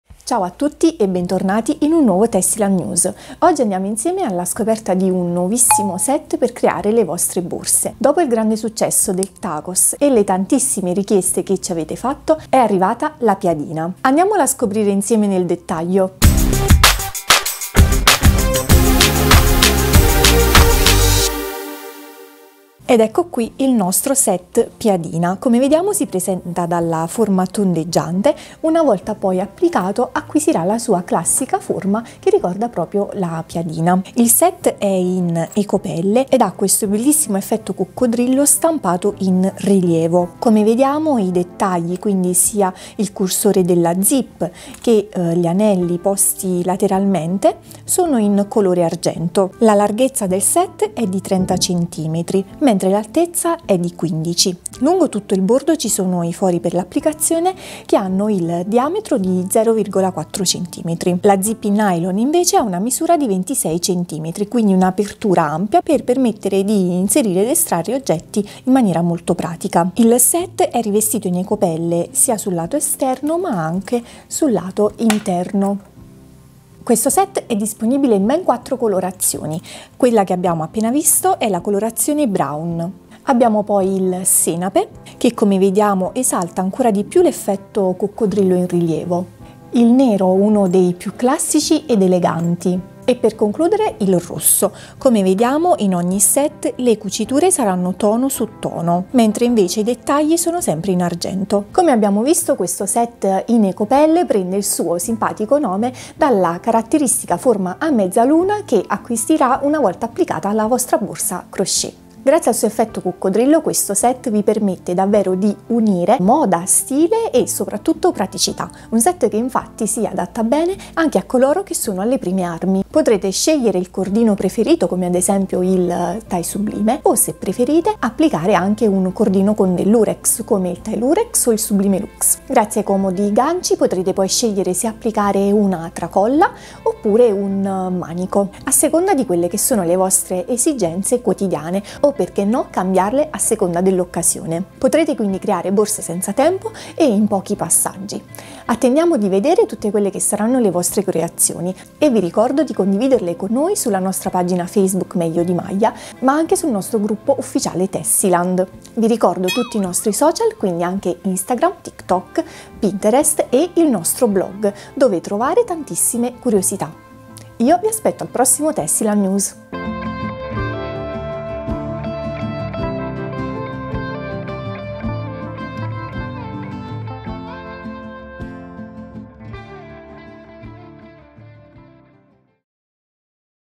Sei una crocheter alla moda? Non perdere il nuovo Set Piadina | Tessiland News
Set Piadina in Ecopelle: Eleganza Tonda e Pratica

Ispirato dalla classica forma della piadina, il Set Piadina in ecopelle è un mix di stile e funzionalità. Con la sua forma tondeggiante e dettagli argentei, rappresenta l'equilibrio perfetto tra estetica e praticità.

Dettagli Che Fanno la Differenza
Largo 30 cm e alto 15 cm, il Set Piadina è adornato con una zip in nylon a spirale lunga 26 cm, rinforzata con cuciture impeccabili. La rivestitura interna ed esterna in ecopelle garantisce durata e una finitura di alta qualità.